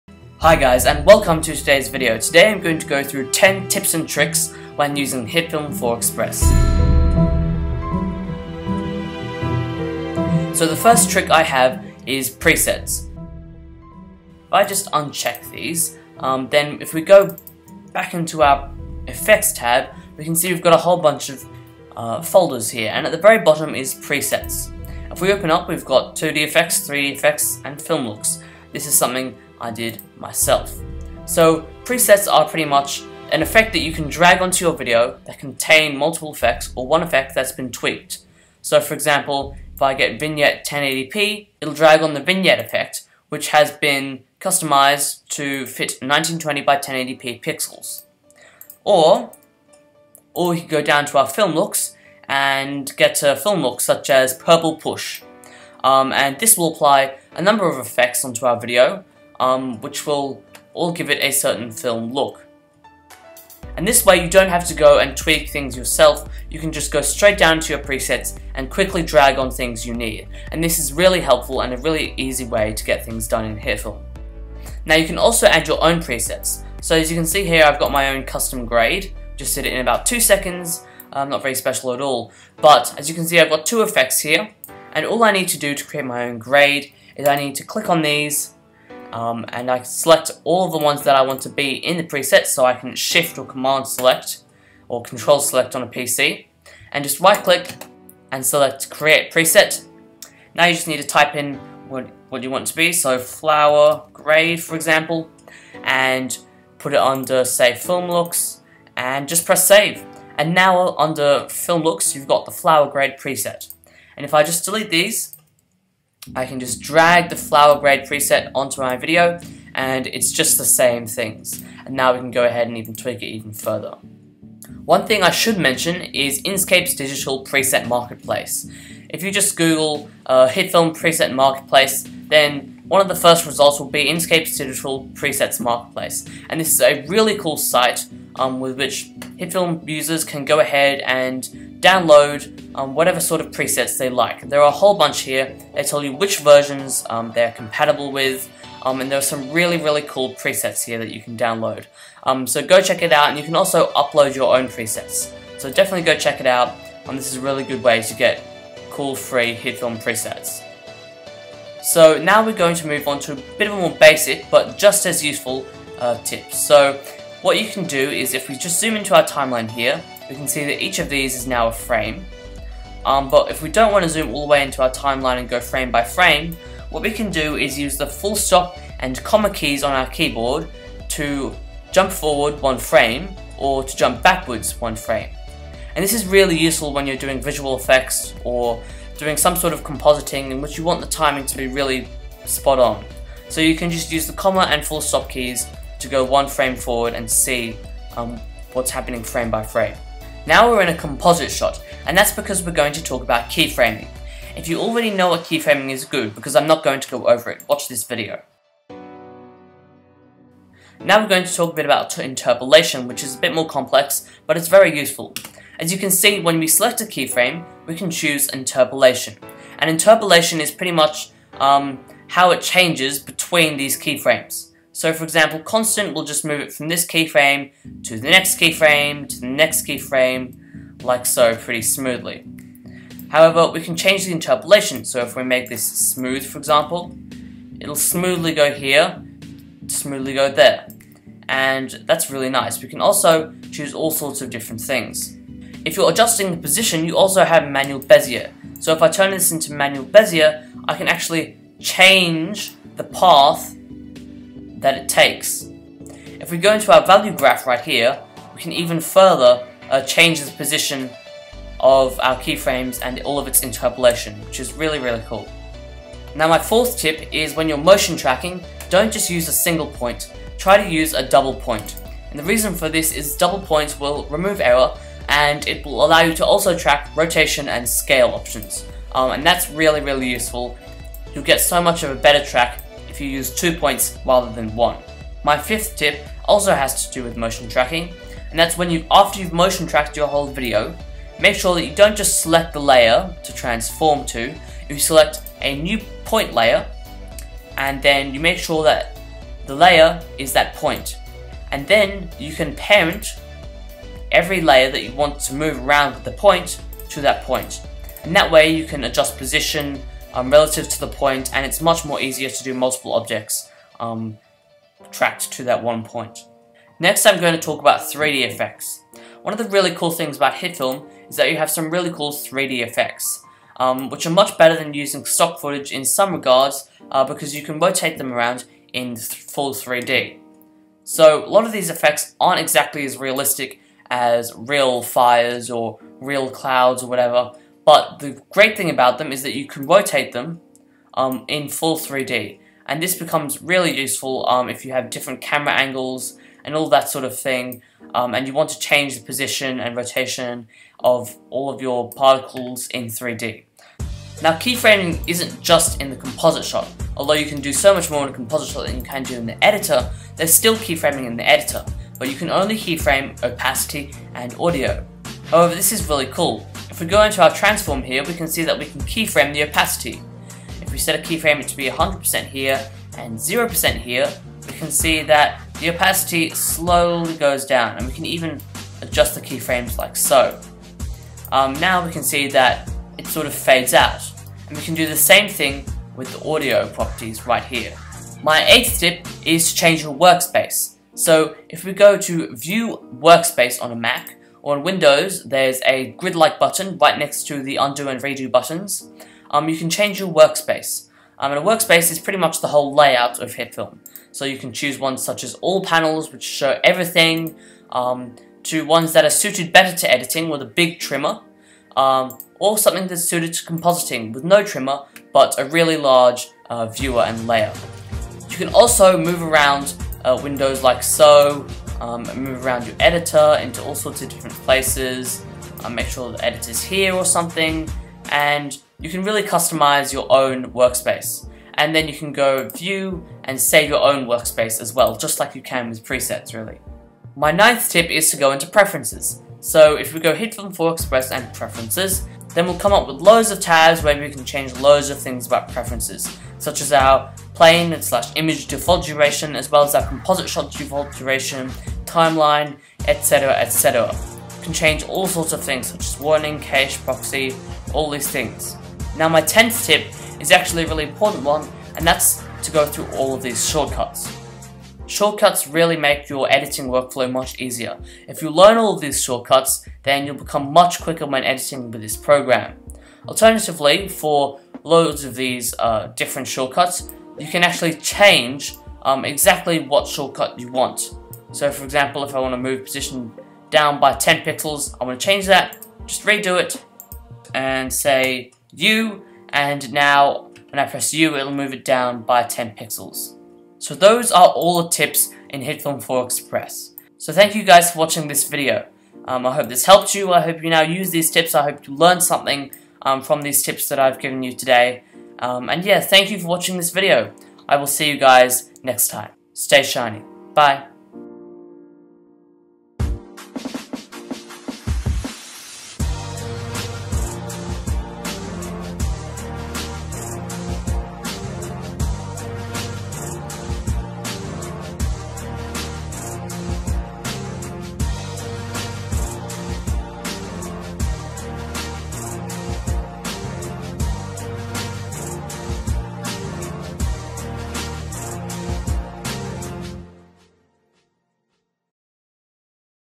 10 Hitfilm 4 Express Tips & Tricks!
Here are 10 Tips and Tricks to help you as you edit your videos for free using Hitfilm 4 Express. Take a look at some of my other videos which explain these aspects of Hitfilm in greater detail.